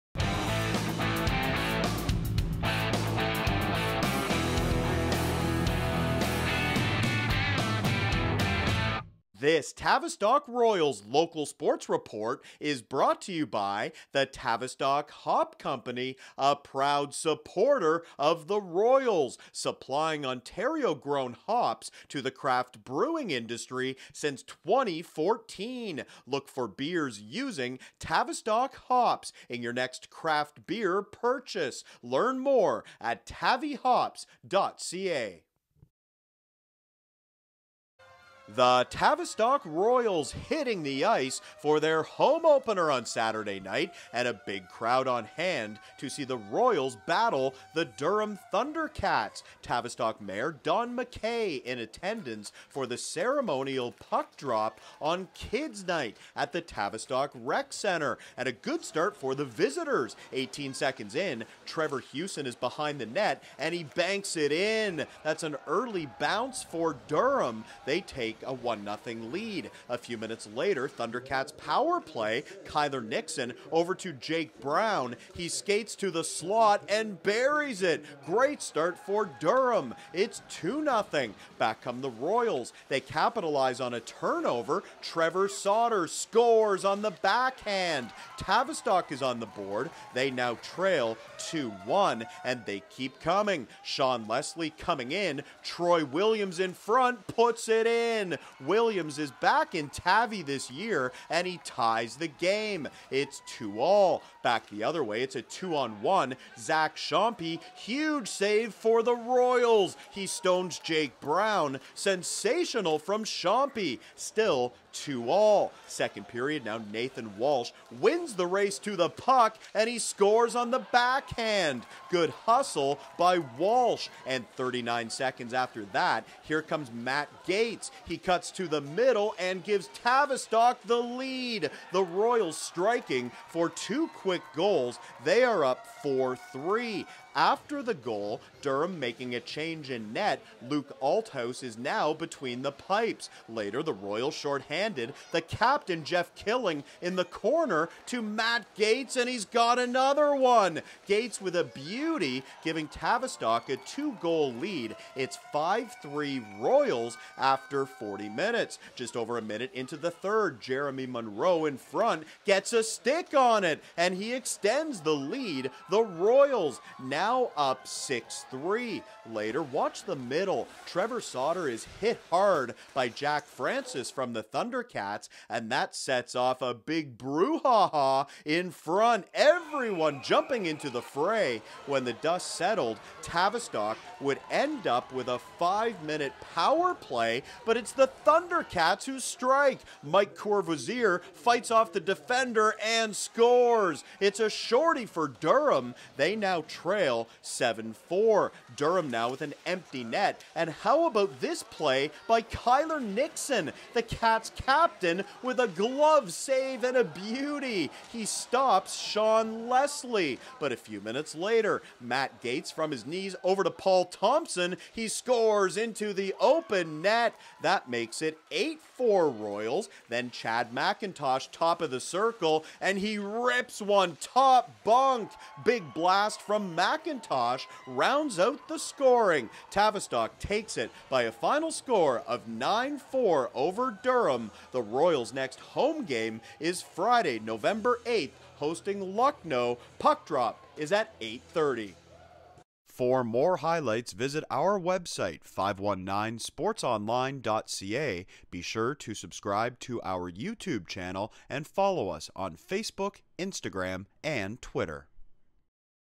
WOAA Hockey - Durham Thundercats vs Tavistock Royals
Highlights from Saturday October 26th between the Thundercats and Royals in Tavistock.